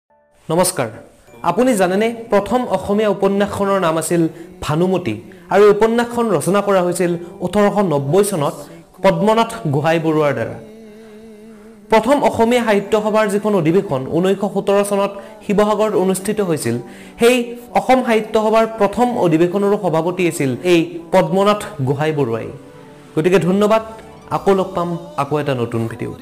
First assamese novel Bhanumati ।। Padmanath gohai baruah
This video is about first assamese novel Bhanumati.This novel was written by eminent assamese writer, activest, freedomfighter Padmanath gohai baruah.Baruah was the first president of Assam sahitya sabha